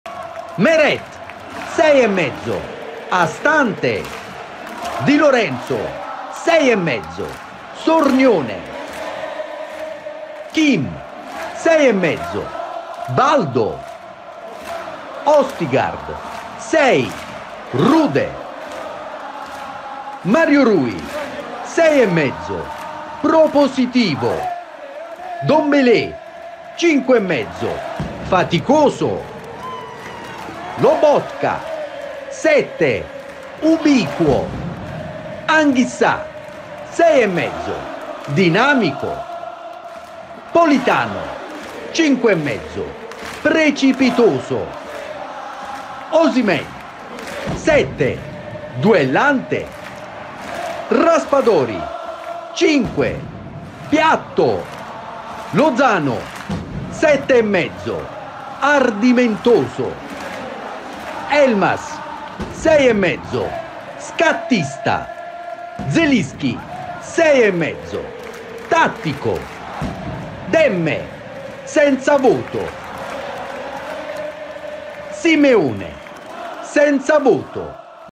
NAPOLI EMPOLI 2-0, IL PAGELLONE DELLA PARTITA VOTI E GIUDIZI
Finisce 2-0 al Maradona tra Napoli ed Empoli. Partita più complicata del previsto per gli azzurri che con i cambi di Spalletti riescono a sbloccare il risultato nel secondo tempo. Il pagellone di Alberto Canonico boccia i 3 usciti all'inizio della ripresa.